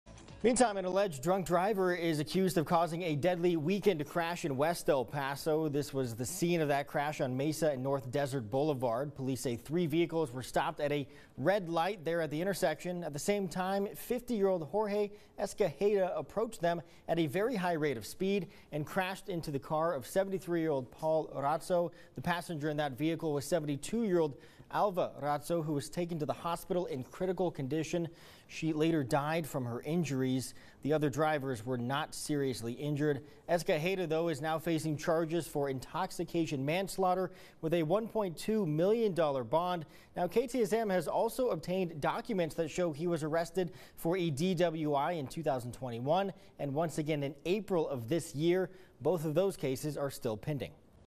1 dead, 3 injured after multi-vehicle crash in West El Paso
One person is dead, and three others injured after a four-vehicle crash in West El Paso, according to El Paso Police.

And a man is now facing a charge of intoxication manslaughter, police said.